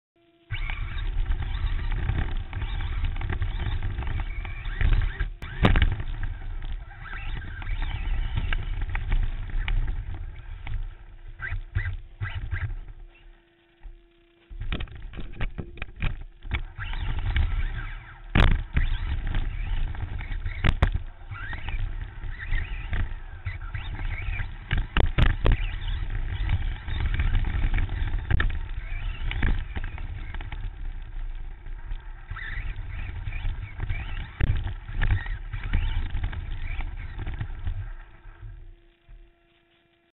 Electric Pit Raceway Mod SCT B Main Restart (GoPro Hero Wide Camera) 3-16-11
Electric Pit Raceway
Mod SCT B Main Restart
3-16-11

GoPro Hero Wide Camera installed in a Team Associated SC10.

The B Main wsa restarted due to bumpers get locked up and creating a pileup.  Got to love SCTs! :)